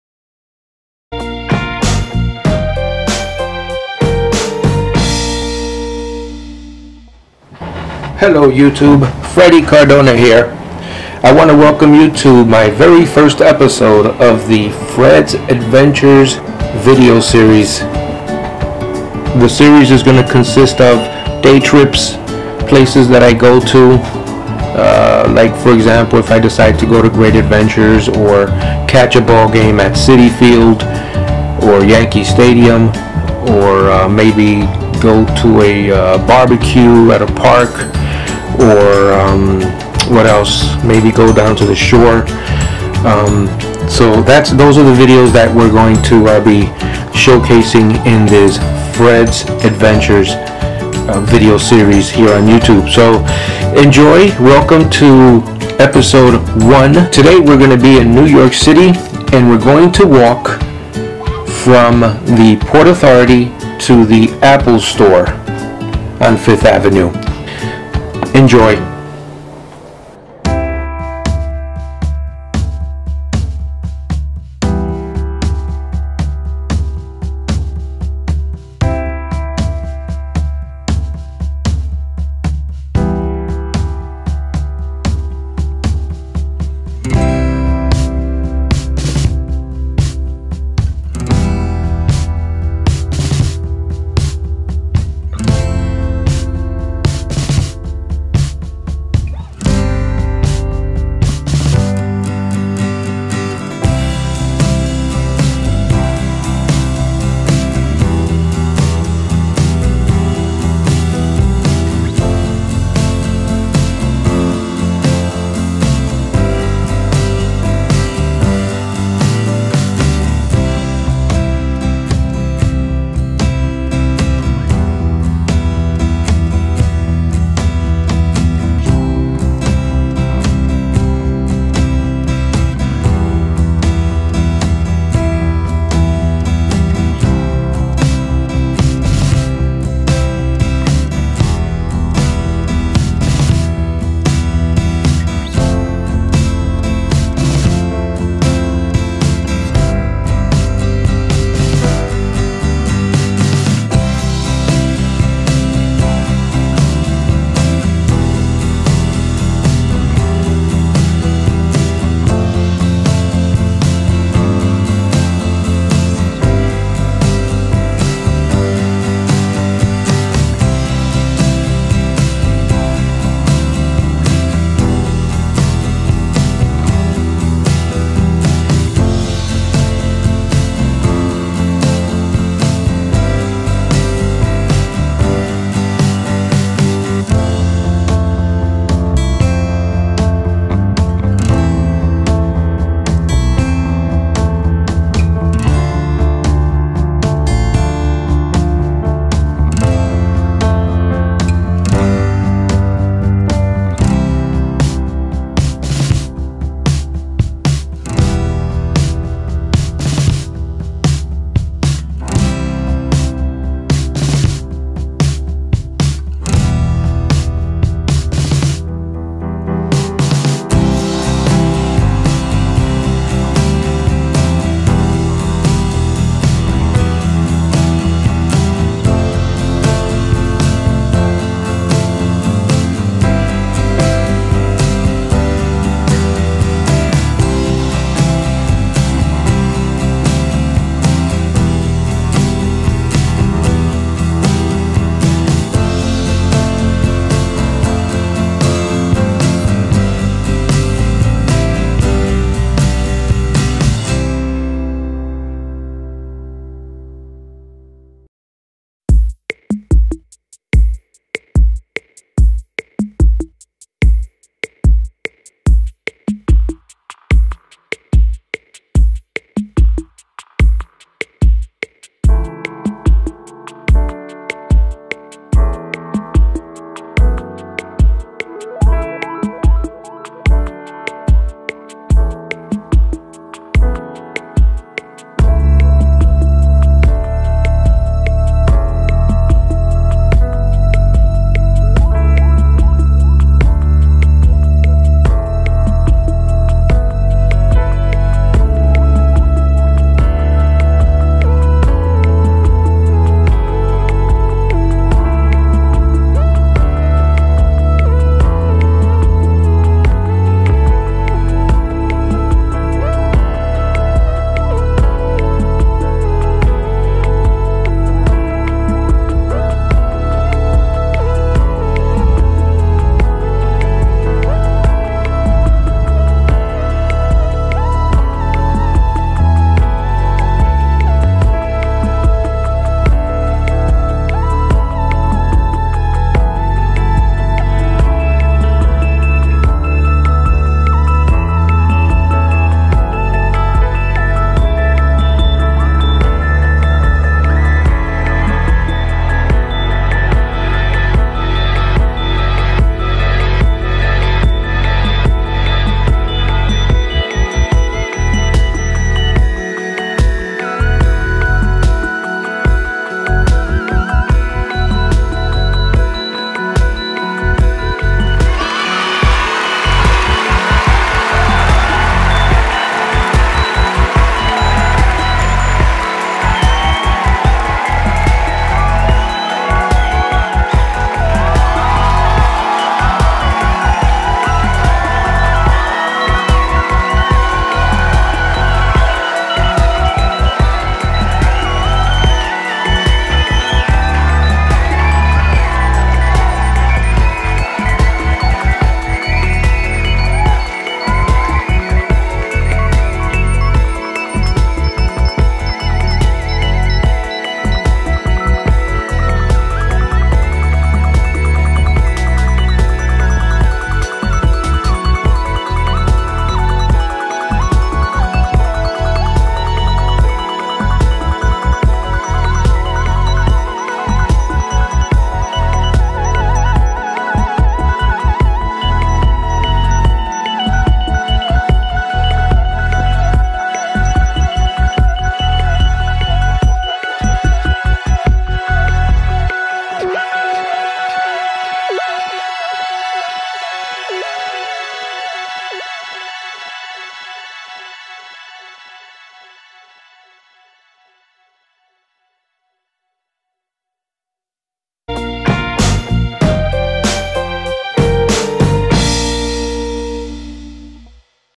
Fred's travels: Episode 01 NYC walking to the apple store
hello subscribers! welcome to the very first episode in a new series called "Fred's Adventures".  in this series, I bring you with me as I visit tourist locations and other places! In this episode we walk from the Port Authority to the Apple Store in New York City.